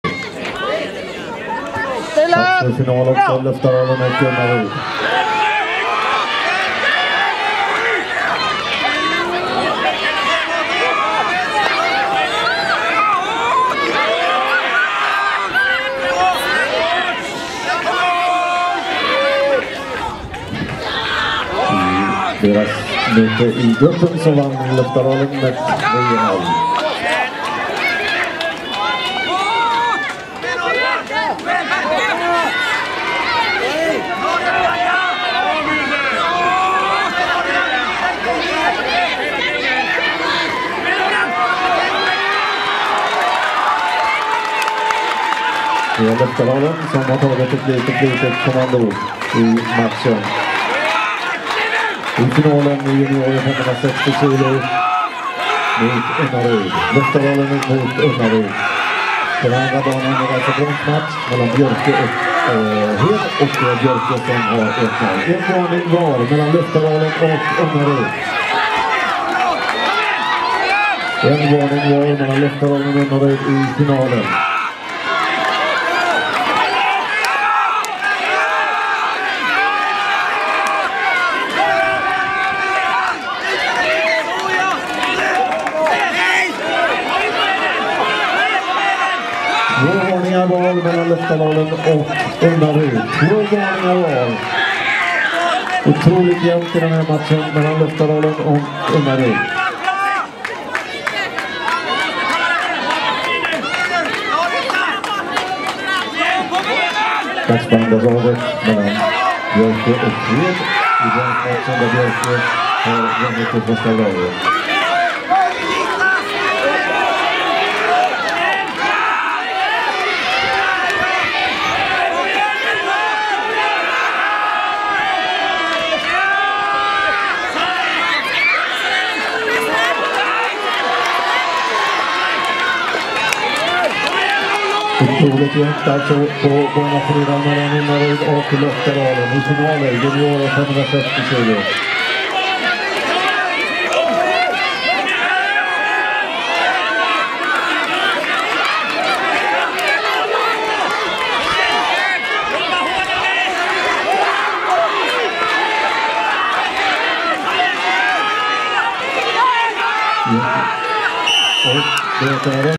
SM i Frillesås 2012 - Ungdom560 FINAL del 1 (Unnaryd vs. Löftadalen)
SM-GULD TILL UNNARYDS UNGDOMAR!